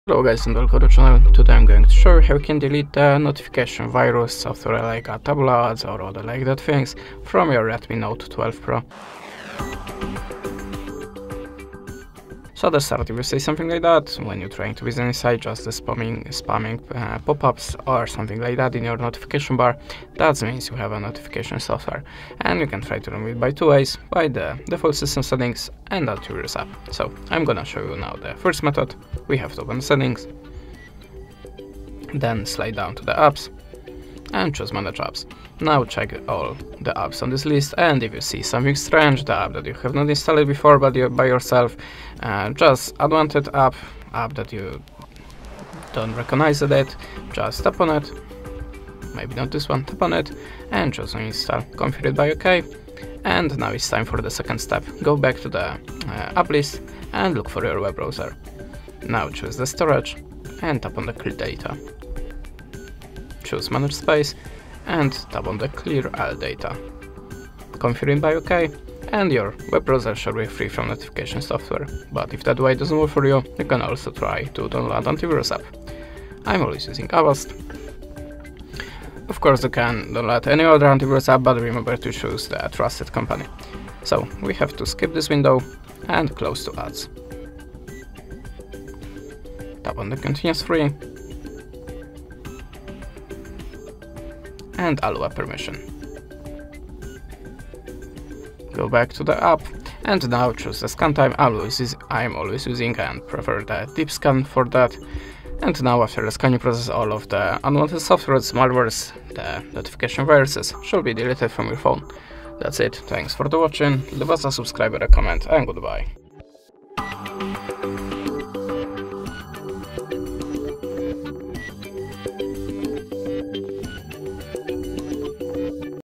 How to Delete Virus Notifications on Redmi Note 12 Pro?
In this tutorial video, you will learn how to easily remove virus notifications from your Redmi Note 12 Pro smartphone. Follow the step-by-step guide and protect your device from harmful viruses and malware.

How to Remove Virus Notifications on Redmi Note 12 Pro?